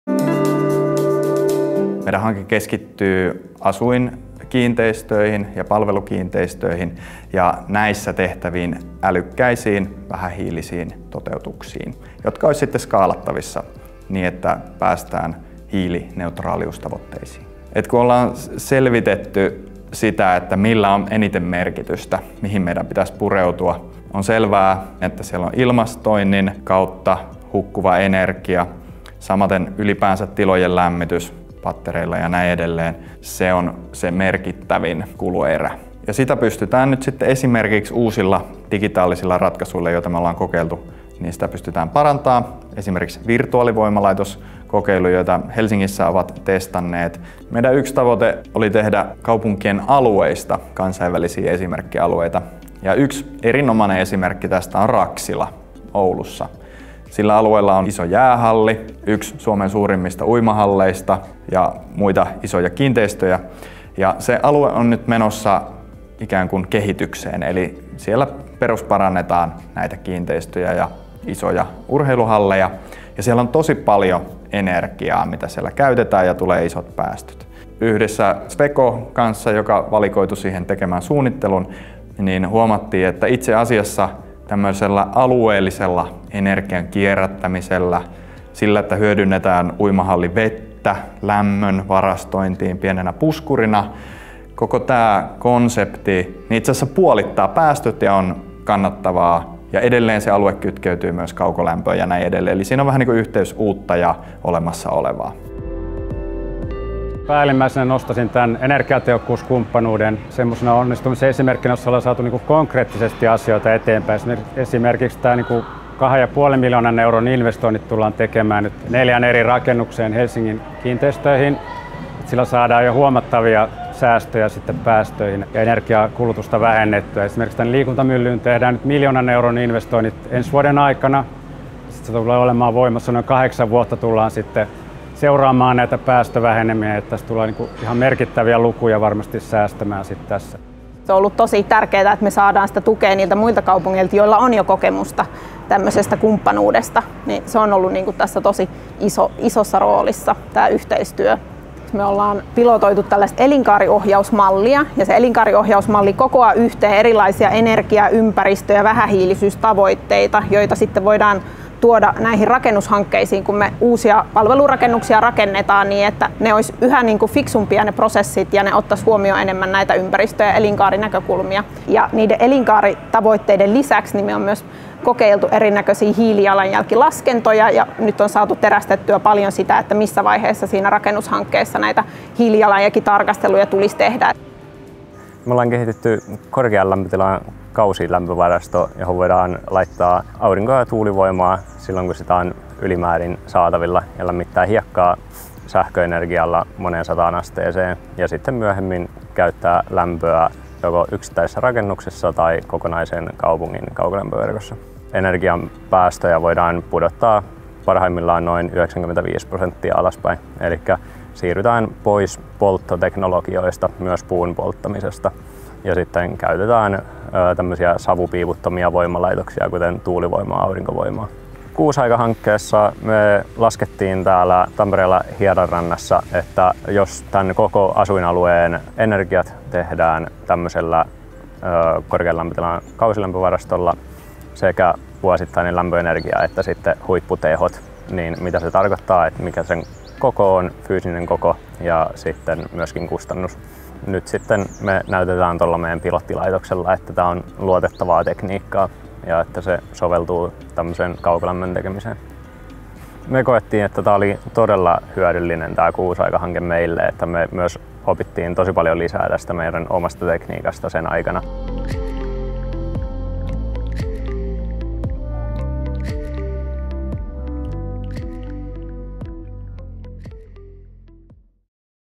Uusia energiaviisaita ratkaisuja kaupunkeihin**New energy-wise solutions in the cities
Kaikkien kuutoskaupunkien yhteinen Energiaviisaat kaupungit -hanke kehittää uusia ratkaisuja kaupunkien asuin- ja palvelukiinteistöjen energiatehokkuuden parantamiseksi. Haastattelussa projektipäällikkö Tuomas Vanhanen Tampereen kaupungilta ja Helsingin osahankkeesta vastaavat Timo Määttä ja Emma Berg. Tommi Eronen Polar Night Energystä kertoo, miten yritys on ollut mukana hankkeessa ja mitä se on siitä hyötynyt.

The Energy Wise Cities project has developed new, energy-efficient solutions for residential and service buildings in the Six Cities. Interviews with Project Manager Tuomas Vanhanen, City of Tampere, Project Manager Timo Määttä and Project Coordinator Emma Berg from the City of Helsinki. CEO Tommi Eronen describes the advantages the project has given to Polar Night Energy.

The Energy Wise Cities project is part of the Six City Strategy